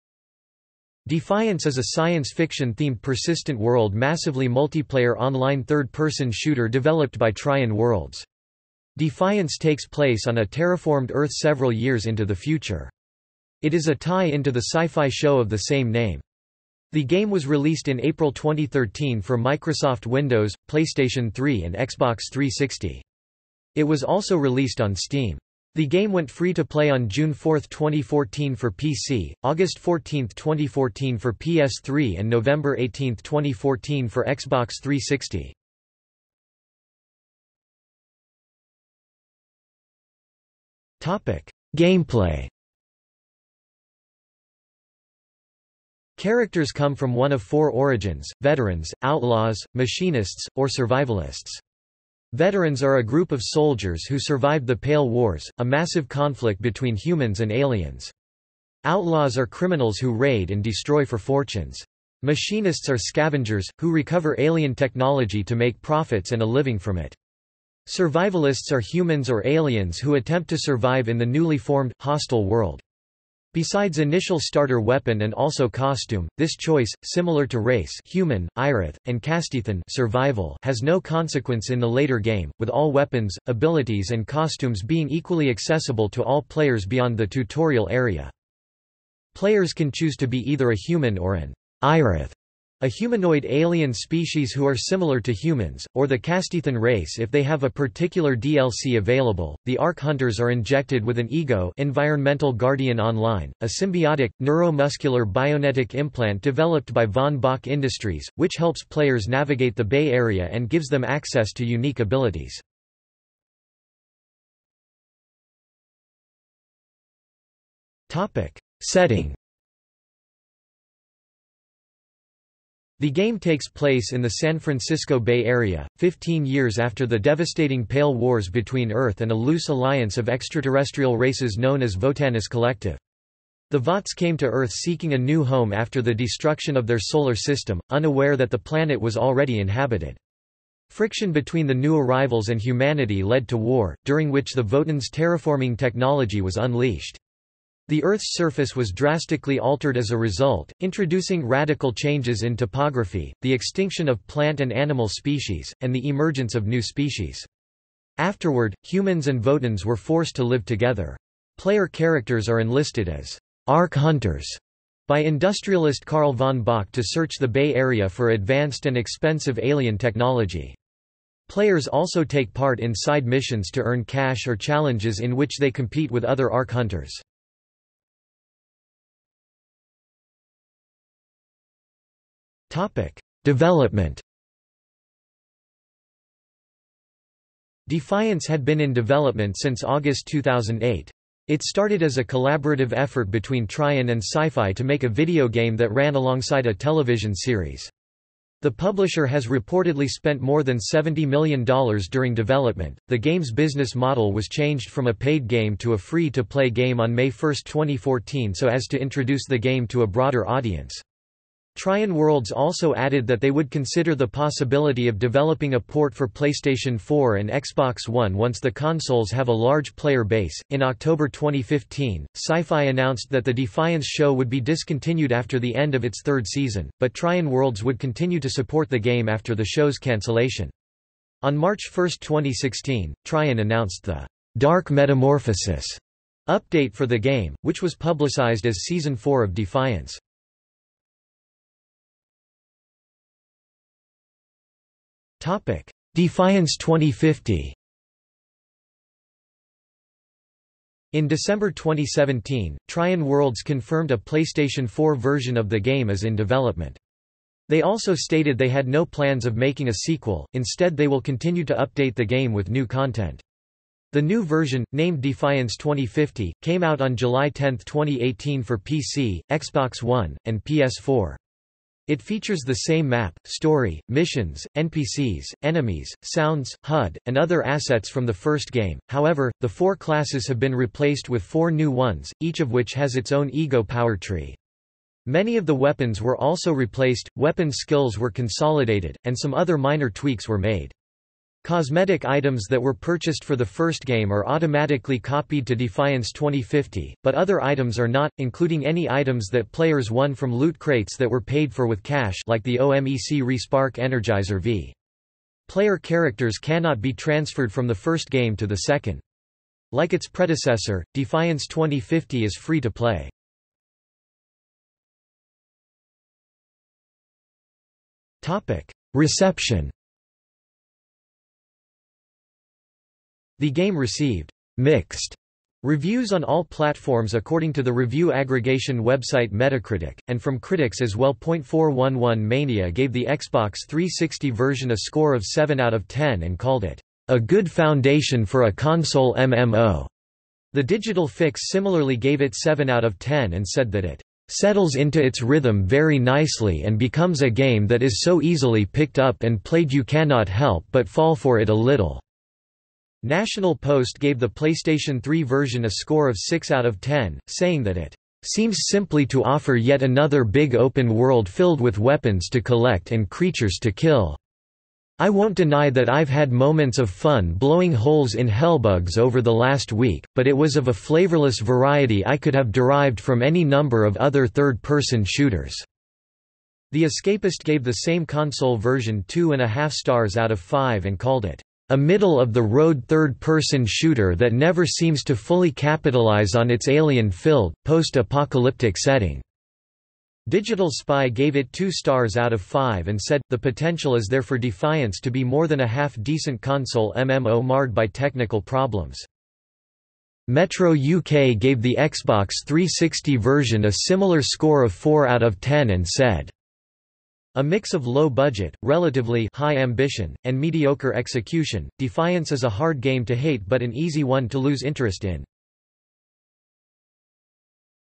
Defiance (video game) | Wikipedia audio article
00:00:37 1 Gameplay
00:01:54 2 Setting
00:02:59 3 Development
00:04:02 3.1 iDefiance 2050/i
00:05:14 4 Reception

- Socrates

SUMMARY
Defiance is a science fiction-themed persistent world massively multiplayer online third-person shooter developed by Trion Worlds. Defiance takes place on a terraformed Earth several years into the future. It is a tie-in to the Syfy show of the same name. The game was released in April 2013 for Microsoft Windows, PlayStation 3 and Xbox 360. It was also released on Steam. The game went free-to-play on June 4, 2014 for PC, August 14, 2014 for PS3 and November 18, 2014 for Xbox 360.